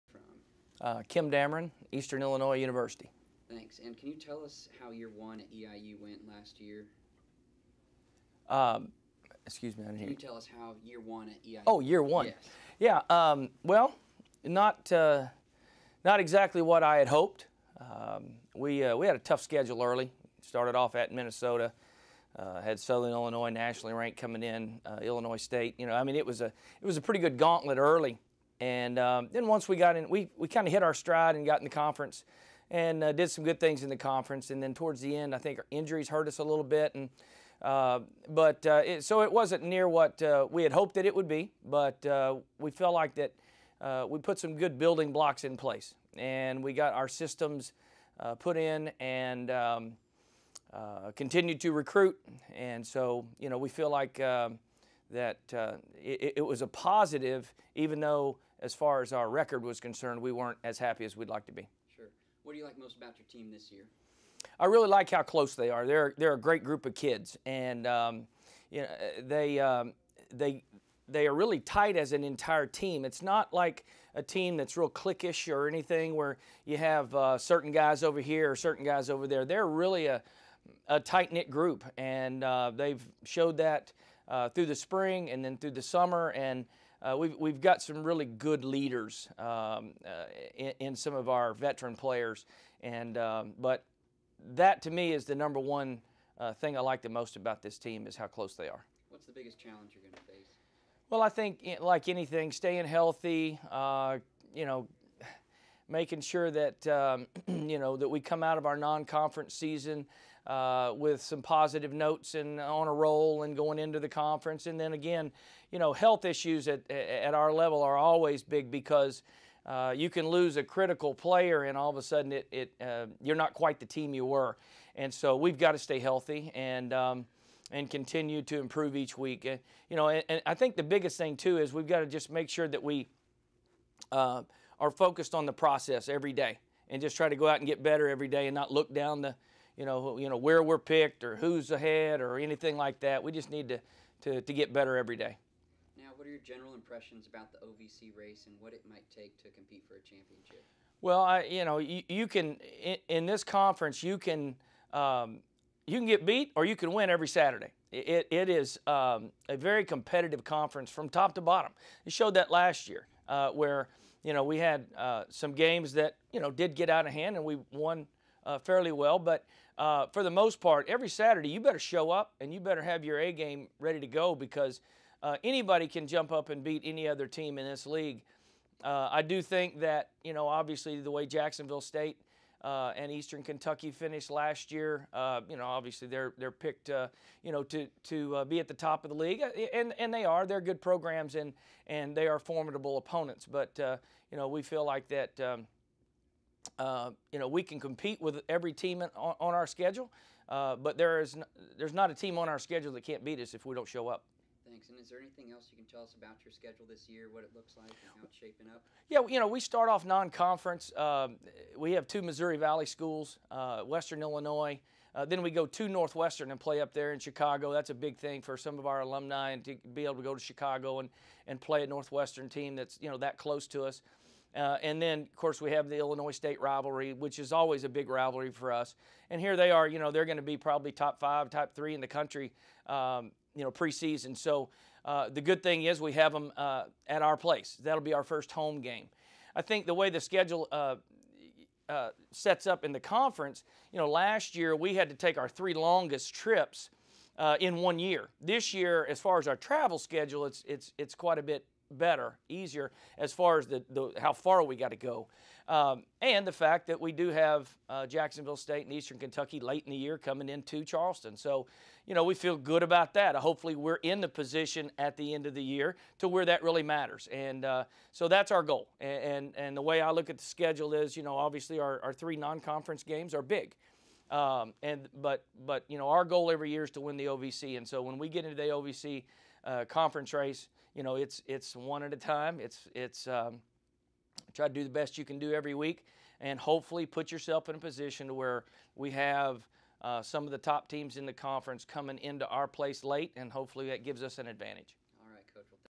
Kim Dameron, Eastern Illinois (2015 OVC Football Media Day)
Eastern Illinois second-year head coach Kim Dameron previews the upcoming season at the recent OVC Football Media Day.